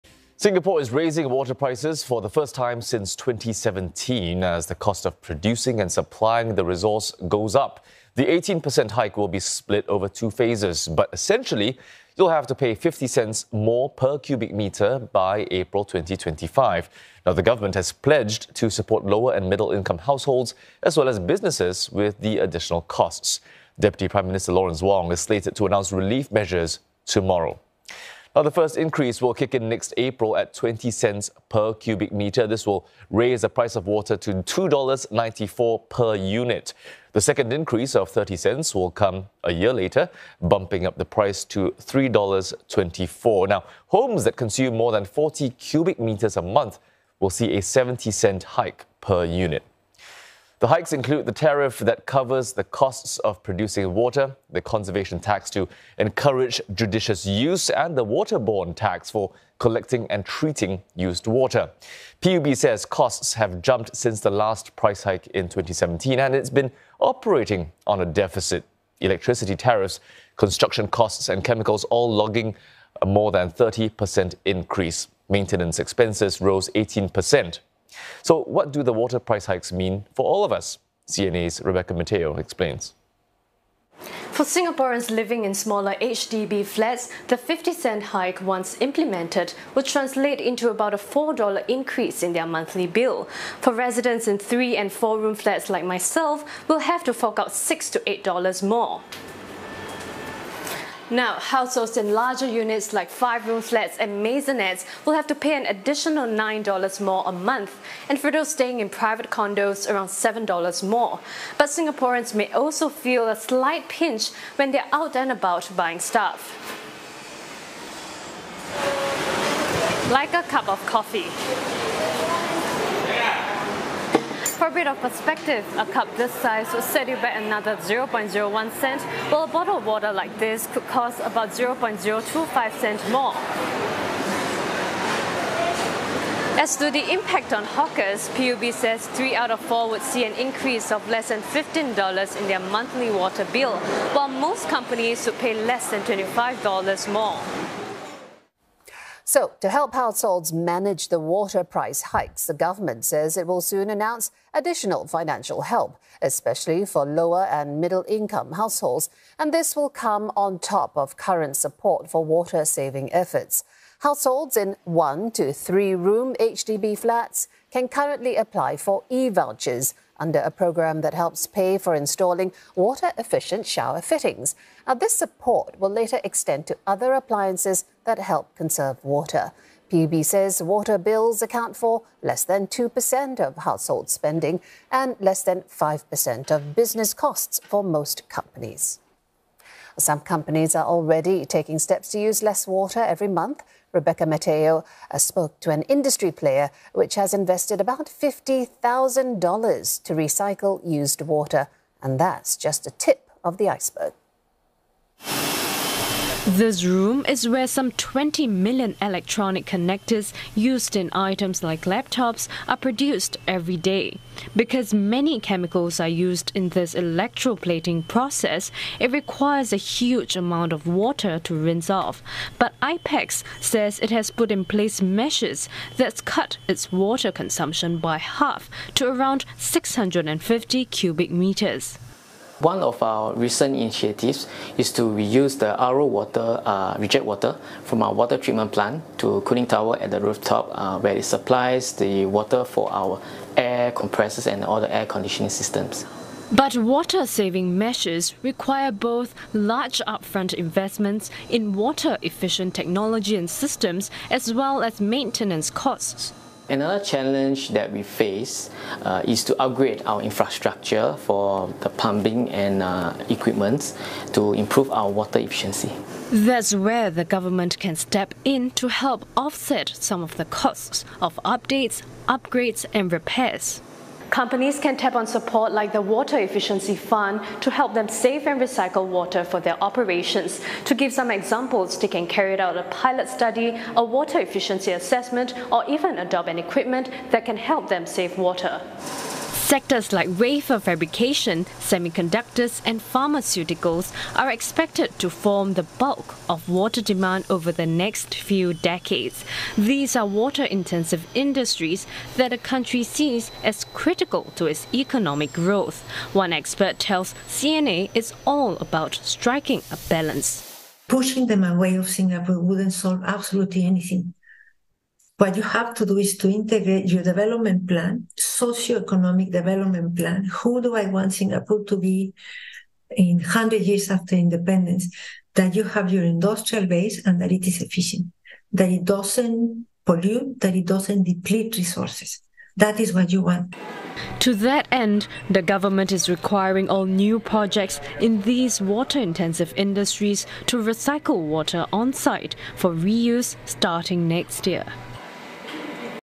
Singapore to raise water prices by about 18% over next two years
Singapore is raising water prices for the first time since 2017. The 18% hike will be split over two phases, with consumers having to pay $0.50 more per cubic metre by April 2025. The first increase will kick in next April at $0.20 per cubic metre, raising the price of water to $2.94 per unit. The second increase of $0.30 will come a year later, bumping up the price to $3.24. The government has pledged to support lower- and middle-income households, as well as businesses, with the additional costs. Deputy Prime Minister Lawrence Wong is slated to announce relief measures tomorrow.